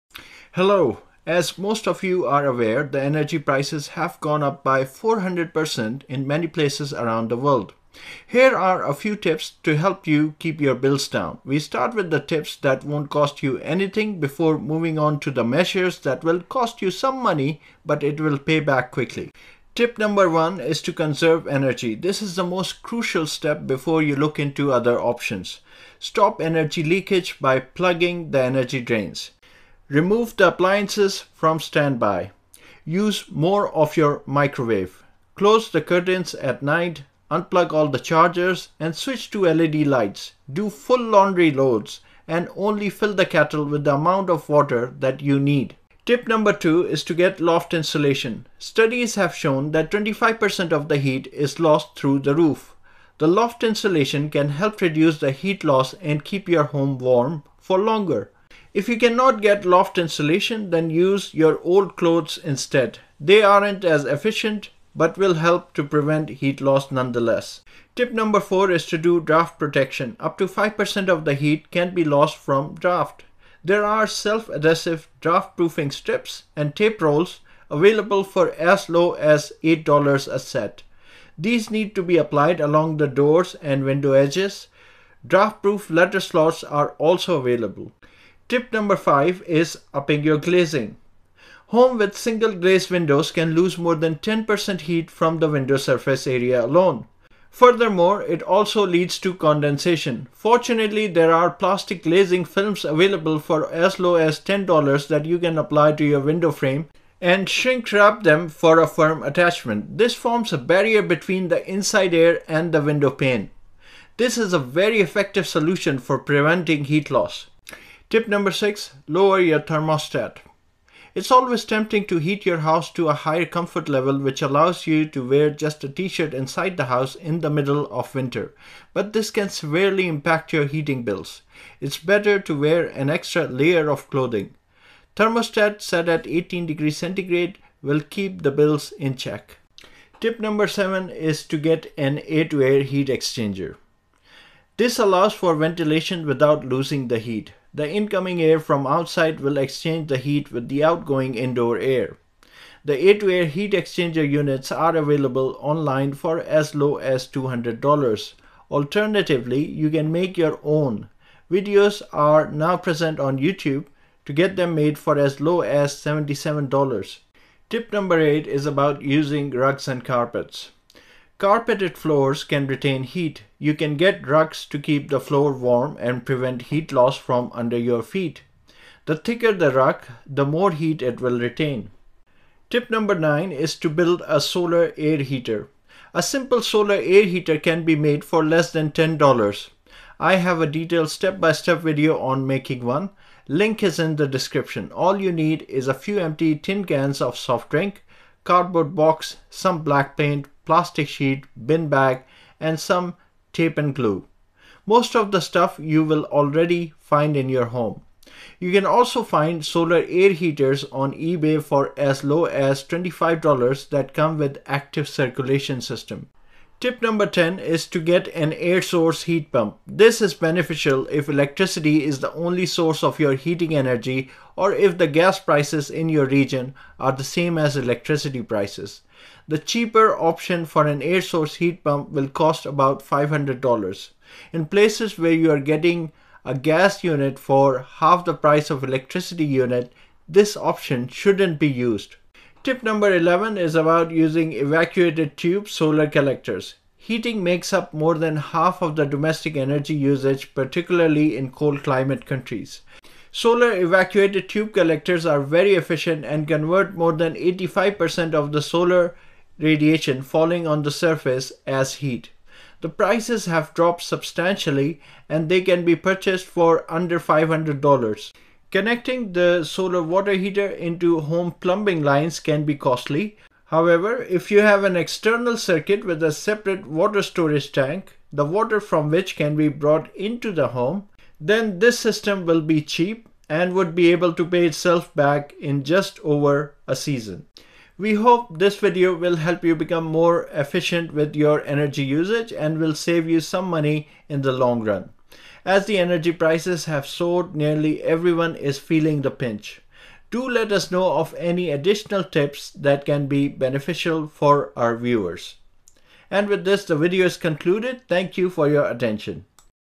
Save thousands on Energy Bills
In this video we give top 10 tips for your to save money on energy bills while still remaining cosy and warm this winter.

Video on making solar air heater